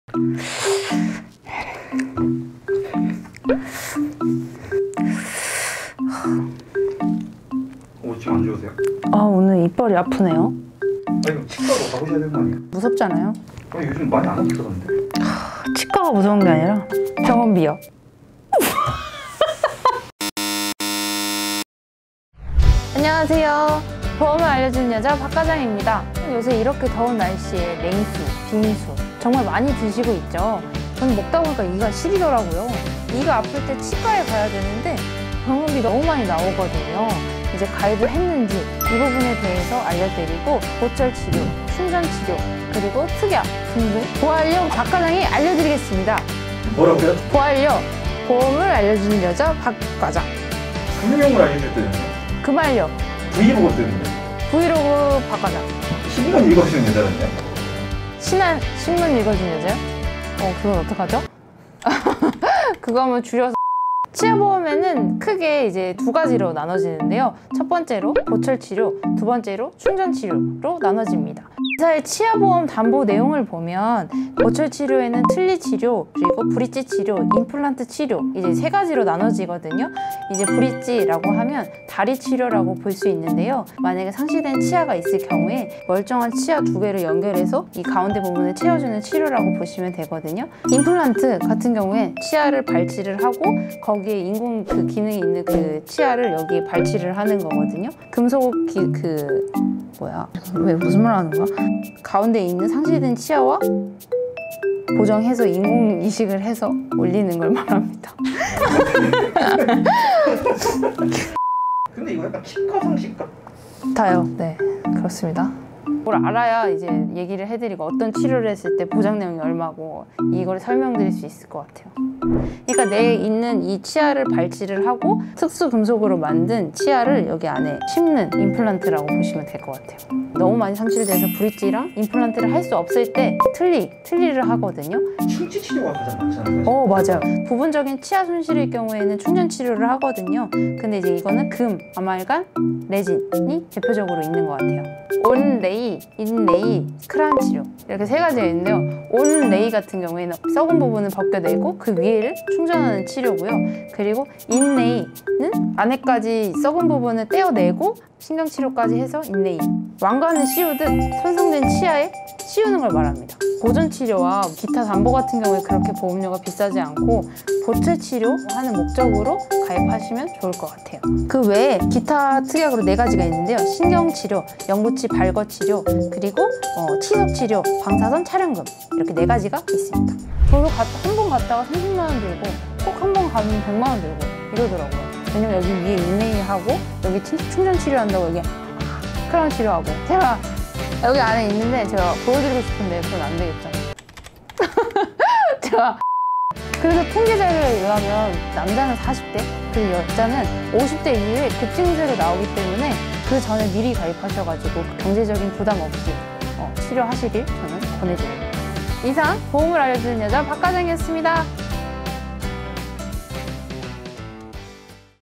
치아보험 , 이건 꼭 알아야 합니다.
치과 가는 것보다 치료비가 더 무서우시다고요?
치아 보험 들기 전 꼭 알아야 할 내용 완벽정리해드리겠습니다!

*원하시는 내용 빠르게 찾아보세요!*
00:00 - 인트로
01:06 - 치아 보험 종류 상식  (보철치료/충전치료)
02:03 - 임플란트 치료
02:25 - 치아 보험 치료방식 (온 레이/인 레이/크라운 치로)
02:53 - 치아 보험 기타 특약